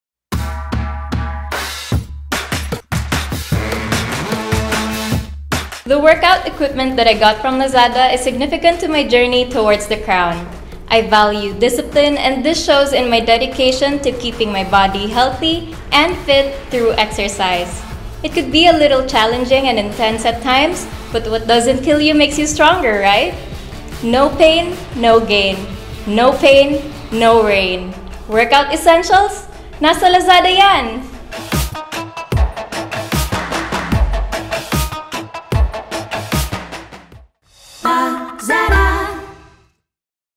Cebu City | Miss Universe Philippines 2021 x Lazada
Can you guess what LazadaPH purchase has helped Bea in her journey to the crown? 👑

Universe, don't forget to VOTE your favorite finalist into the TOP 16! Claim your 5 FREE daily votes now!

Voting ends SEPTEMBER 29, 11:59 PM